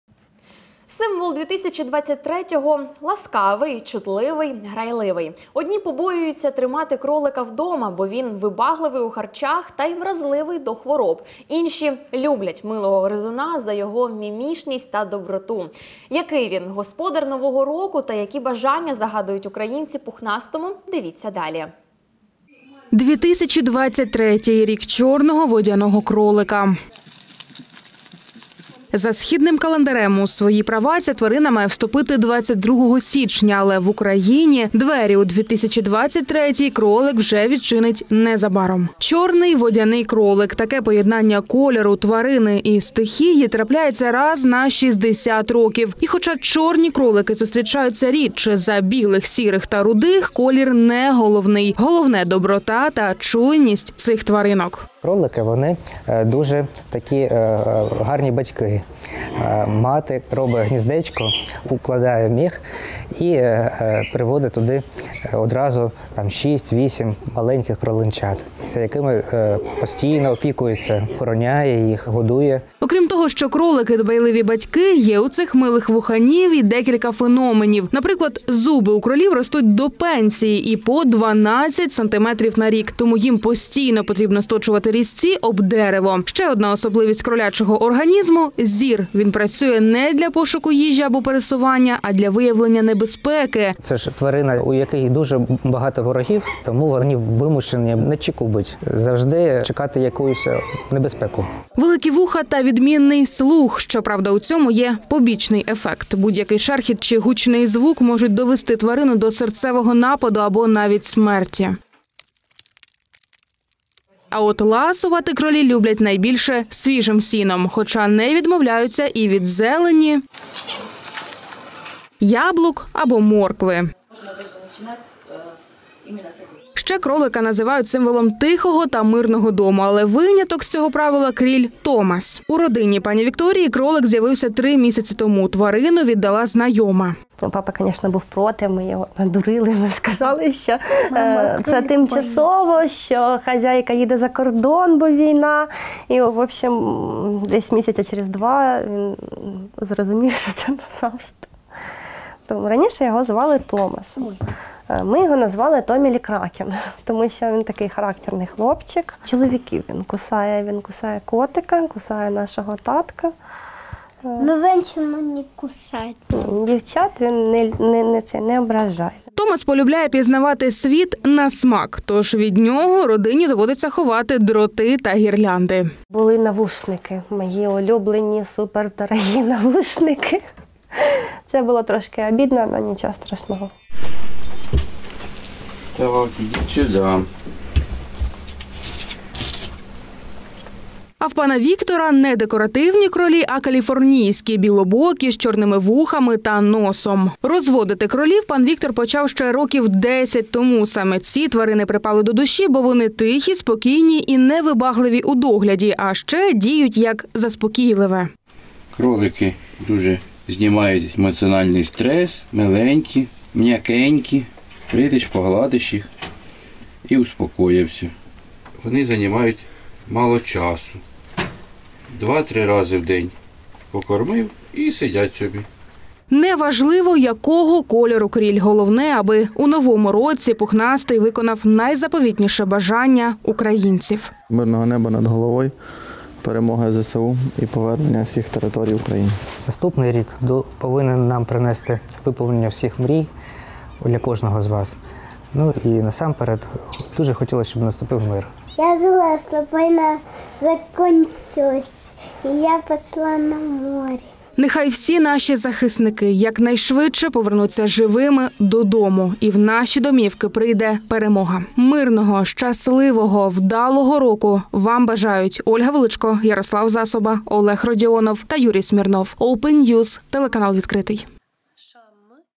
Кролик – символ Нового 2023 року
Ласкавий, чутливий, грайливий. Він – символ 2023 року. Кролик. Одні побоюються тримати цього вуханя вдома, бо він вибагливий у харчах та й вразливий до хвороб. Інші – люблять милого гризуна за його мімішність та доброту. Який він – символ Нового року та які бажання загадують українці пухнастому – дивіться далі. _______________________________________________